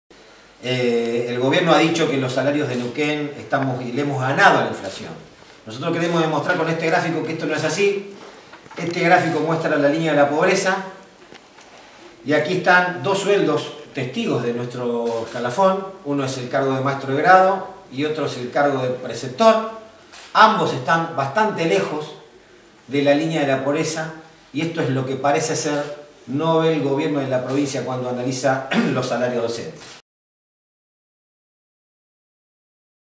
aten Sueldos LineaDePobreza Neuquen
Conferencia de prensa 20/03/2018 del Sindicato Docente Neuquen